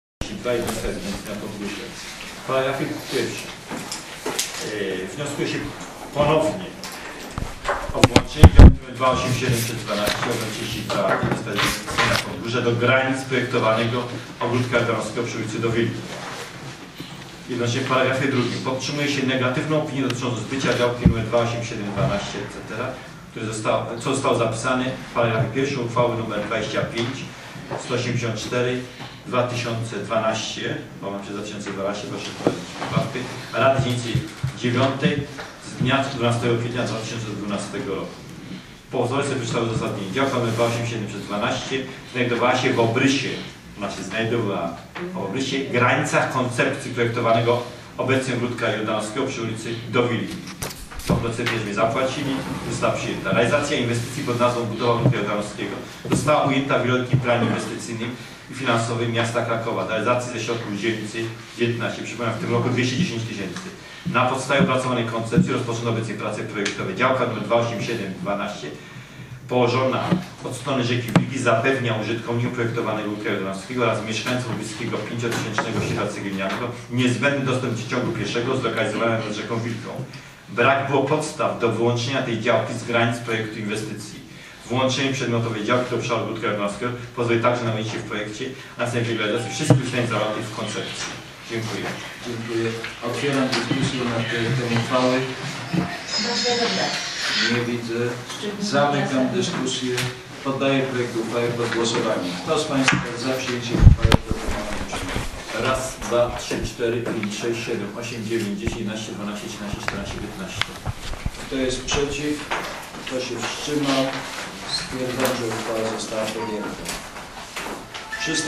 RD IX 28 05 2013 11 10 uchwała w sprawie działki 287 12 obr 32
Obrady XLIX sesji Rady Dzielnicy IX Łagiewniki - Borek Fałęcki z dn. 28 maja 2013 r.